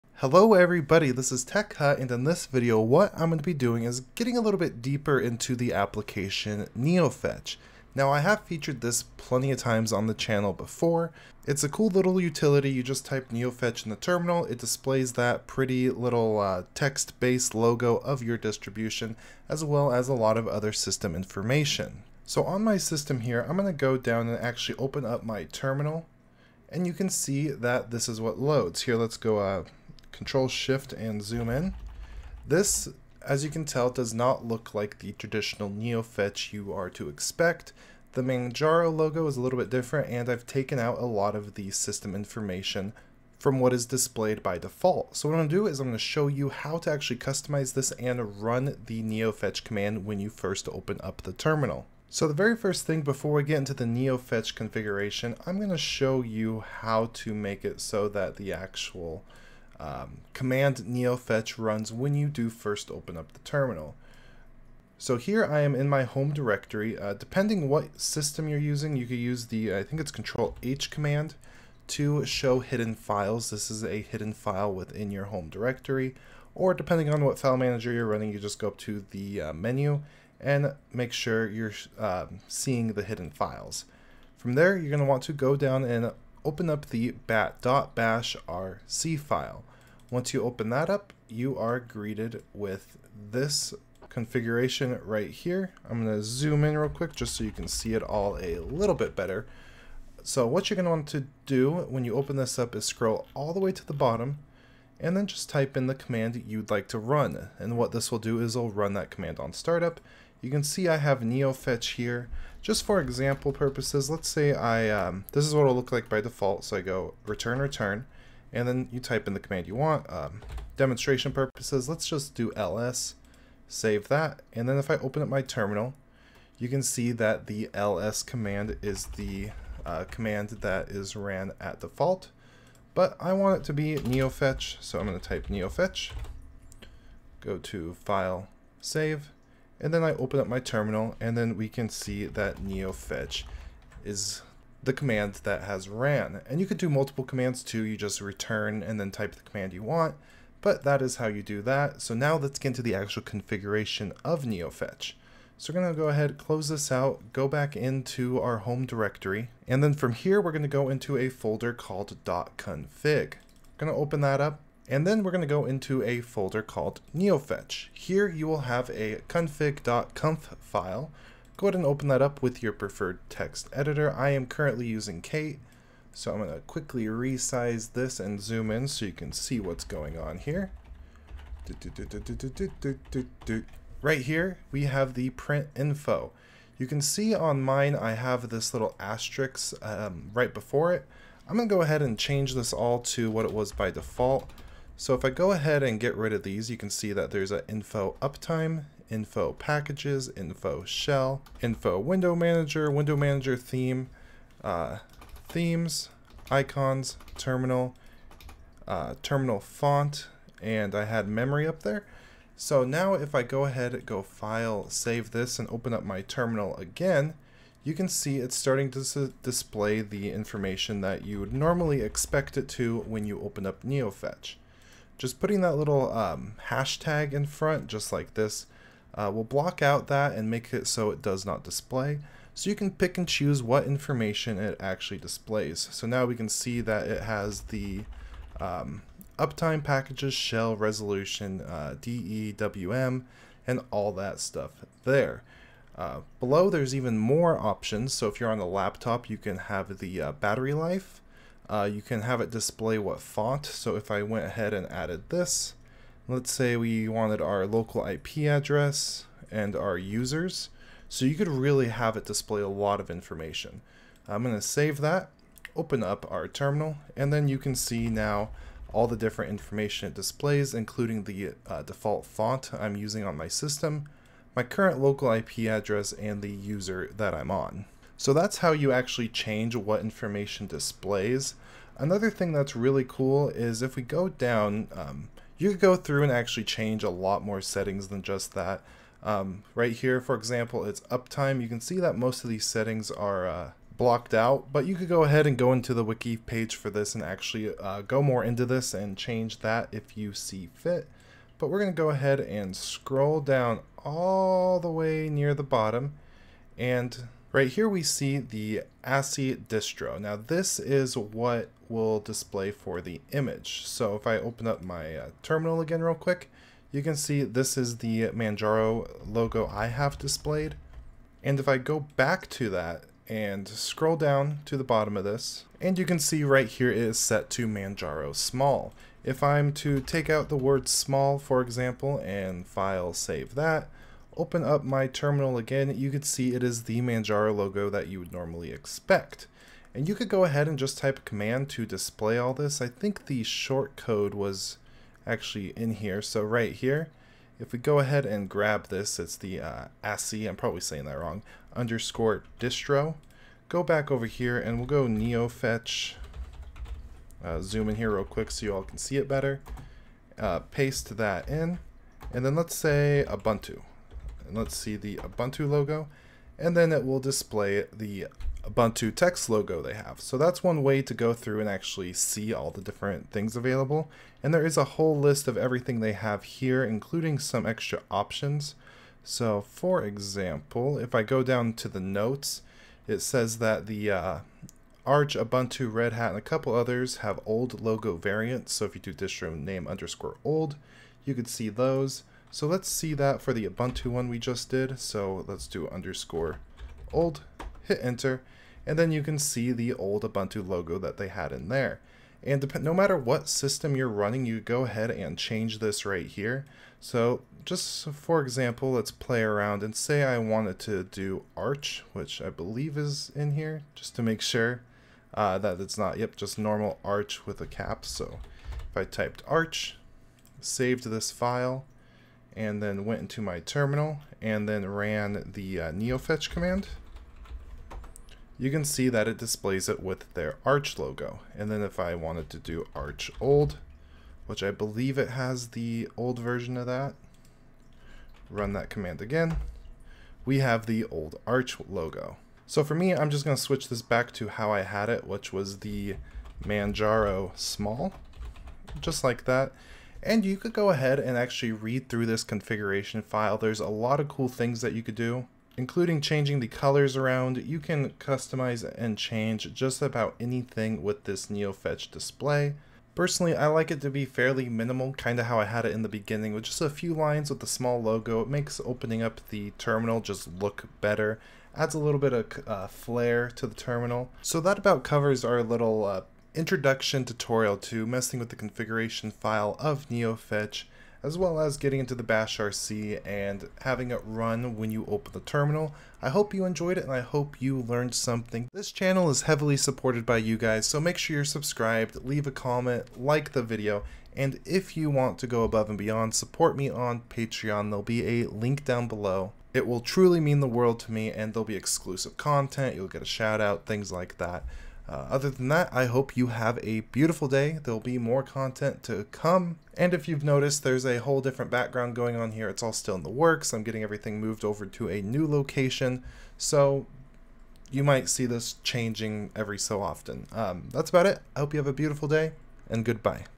Neofetch - Configuration and Customization!
Neofetch automatically launch when you open your Linux terminal will definitely add some flare. We will run down how to do this and some basic configuration options within Neofetch.

"Neofetch is a command-line system information tool written in bash 3.2+. Neofetch displays information about your operating system, software and hardware in an aesthetic and visually pleasing way."